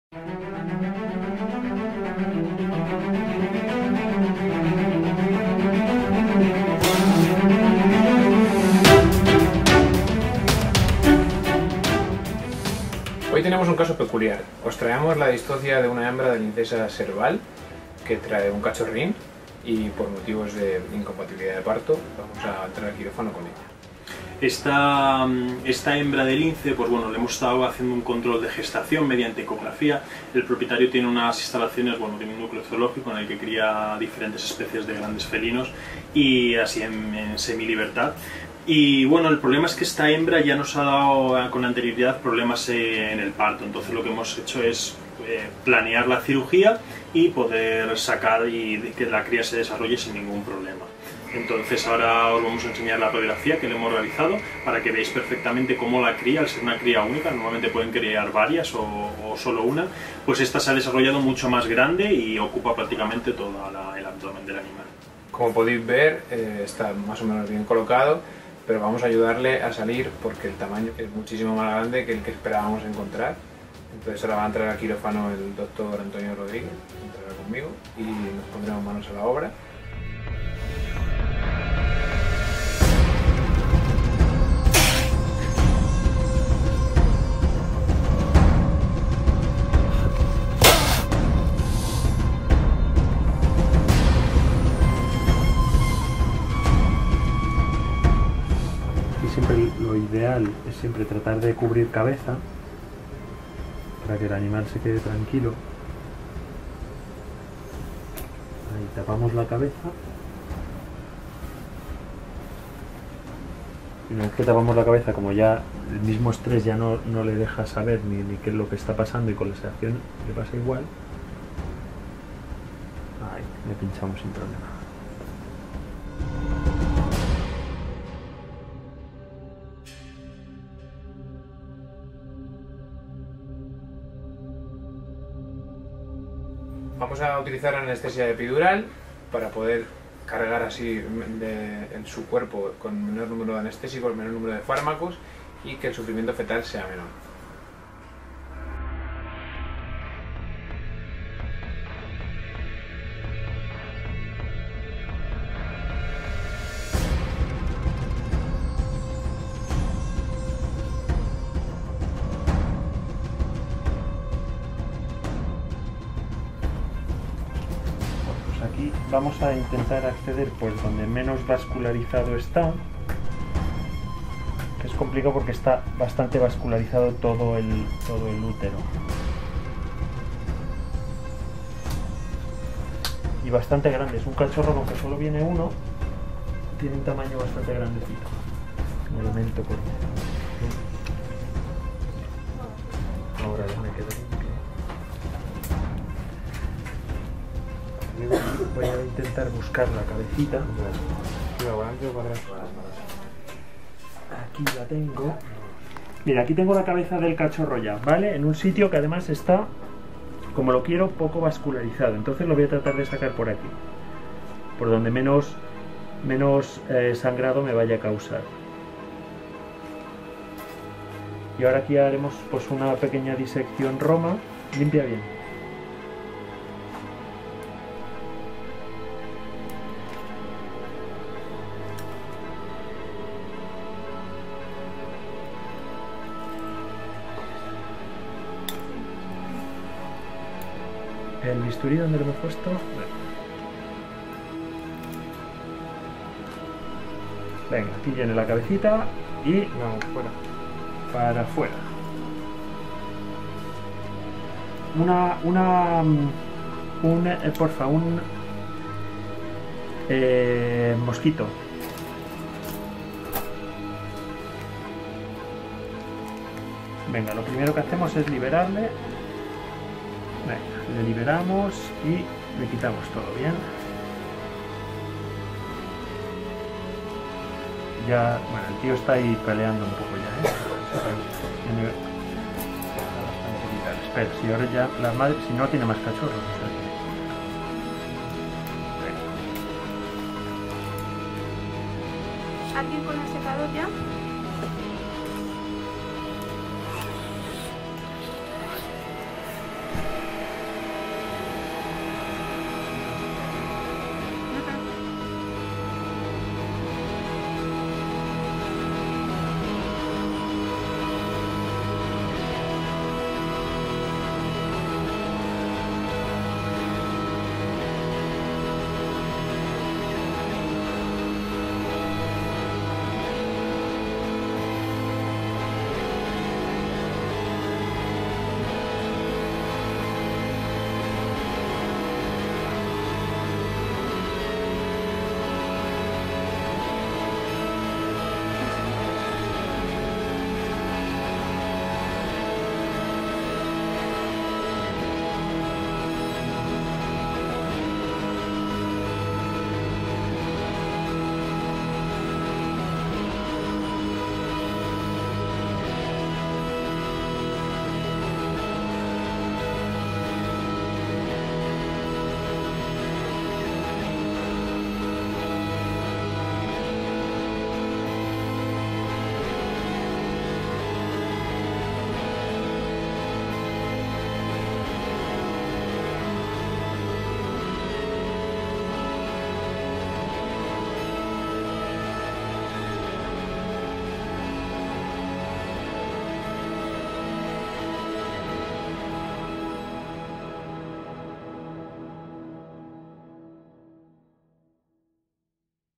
CAP 26 VETERINARIO ONLINE Cesárea de lince Serval. Leptailurus serval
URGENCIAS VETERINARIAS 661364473

Hoy en Veterinario Online, os traemos un capítulo que no os podéis perder. El Dr. Antonio Rodríguez y su equipo de veterinarios operan y sacan adelante una cría de serval tras una cesárea.
Se trata de una especie de felino de tamaño medio, Leptailurus serval. Un mamífero carnívoro que suele pesar de 8 a 18Kg. Es un animal nocturno, por lo que caza sobre todo por la noche, a pesar de estar especializado en roedores, en su dieta también incluye aves, liebres, reptiles, insectos peces y ranas. Tiene un pelaje amarillento con motas negras, la cabeza pequeña y orejas grandes para detectar a sus presas incluso en madrigueras subterráneas. Son buenos corredores, pueden alcanzar una velocidad máxima de 80 km/h y pueden escalar bastante bien. Cuenta con patas largas (el niñas largo de todos los gatos, en relación con el tamaño del cuerpo).
En cuanto a su reproducción, la época de apareamiento del serval tiene una duración de hasta cuatro días, y es capaz de dar a luz varias camadas durante todo el año. La gestación dura de 66 a 77 días, y generalmente resulta en dos cachorros, naciendo a veces tan sólo uno o hasta cuatro.
En este caso, se trata de un único cachorro. Los Dr. Pablo Casar y Javier Beltrán nos explican el caso, nos enseñan la radiografía en la que podremos apreciar perfectamente al cachorro que al ser una cría única, se ha desarrollado mucho más y ocupa prácticamente todo el abdomen del animal.
En quirófano, el Dr. especialista en exóticos Antonio Rodríguez se encargará de ayudar a salir a la cría. El Serval se encuentra con anestesia epidural, con el fin de que el sufrimiento fetal sea menor, el Dr. Rodríguez, deberá sacar al animal lo más rápido posible para que este efecto no sea perjudicial en él.
Durante la operación, la Dra. Patricia Garrido, junto con parte del equipo, espera a que la cría sea liberada para comenzar con la reanimación. Evitando que el cachorro se enfríe y ayudándole a respirar en sus primeros minutos de vida.
Las crías pesan alrededor de 250g al nacer, y son al principio ciegas y débiles, abren los ojos a los 9 y 13 días de edad, y comienzan a ingerir alimentos sólidos después de un mes. Aproximadamente a los seis mesas, adquieren sus dientes caninos permanentes y comienzan a cazar por si mismos.
La esperanza de vida de esta especie e de unos 10 años en la naturaleza, y hasta 20 años en instalaciones controladas.
Este animal recibe cuidados de incubadora y se alimenta cada tres horas con leche artificial.